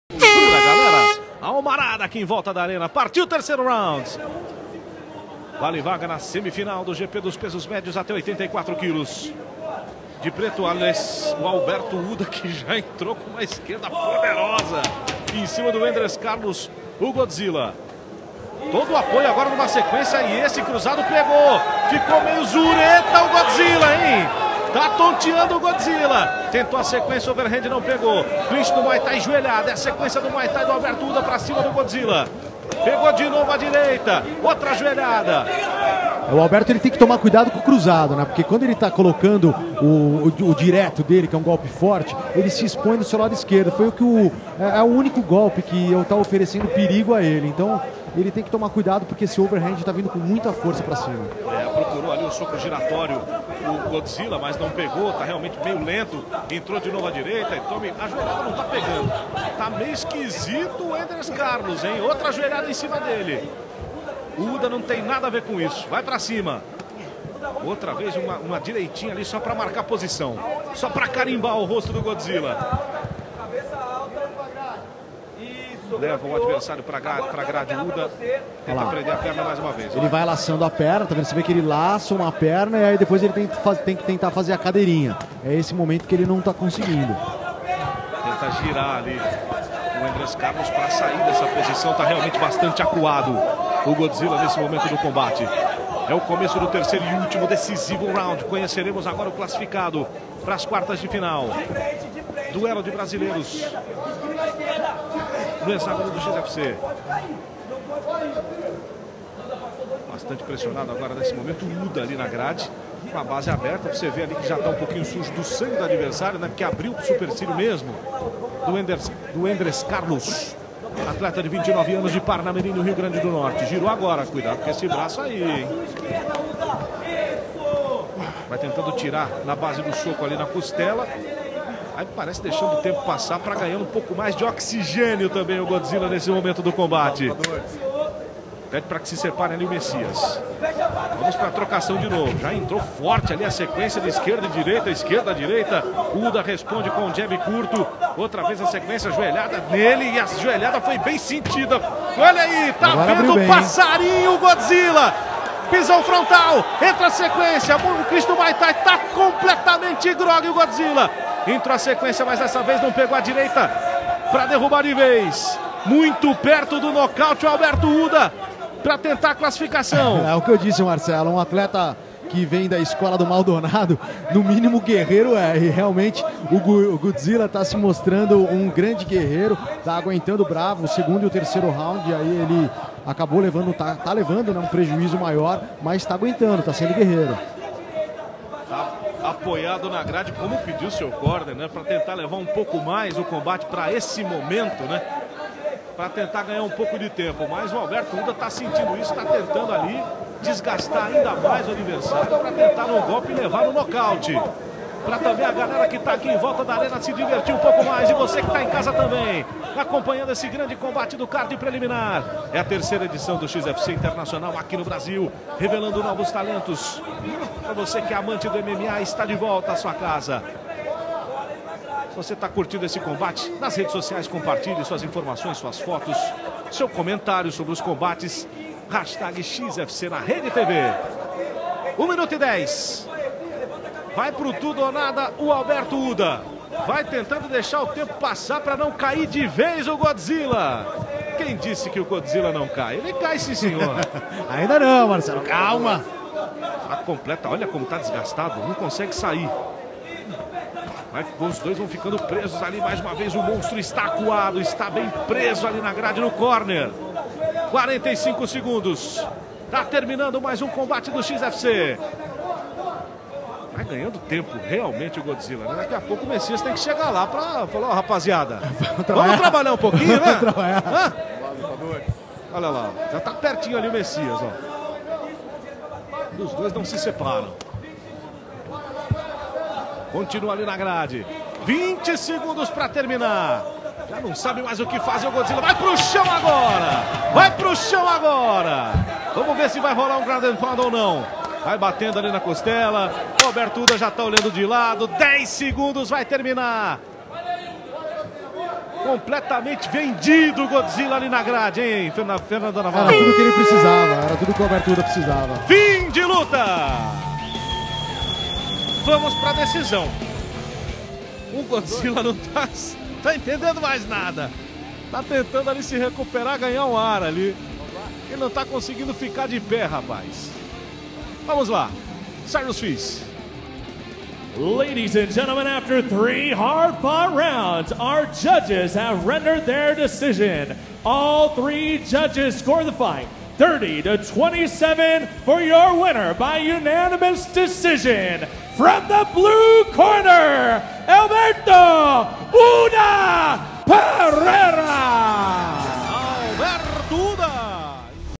XFC na RedeTV!: Alberto Uda vence Wendres Carlos por decisão dos juízes
Depois de três rounds bastante disputados, os juízes decidiram dar a vitória a Uda por unanimidade